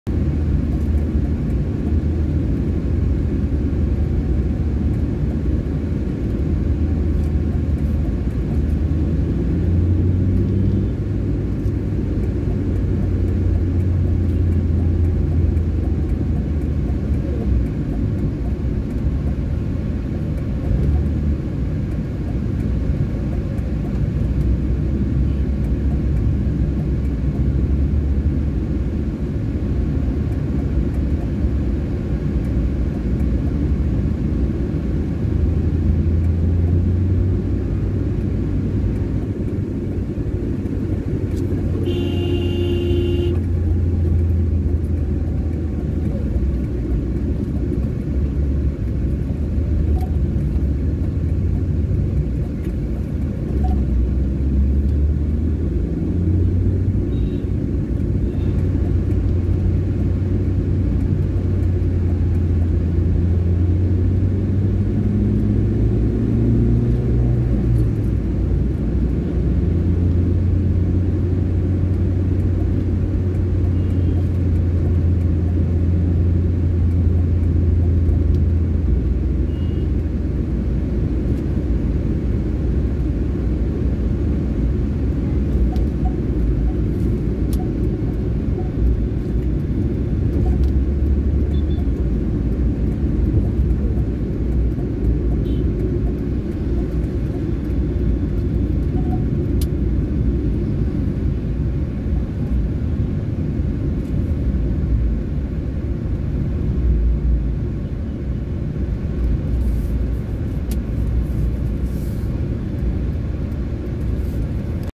Arjangarh To Gurgaon Road Trip - Arjangarh Gurgaon Highway Area - Mg Road 2021
About This Video - Watch out for speed camera's on MG road from delhi to gurgaon after Chatarpur Metro Station

Topic Cover - SPEED CAMERA ON MG ROAD | DELHI TO GURGAON | THE GALLERY | ARJANGARH | METRO STATION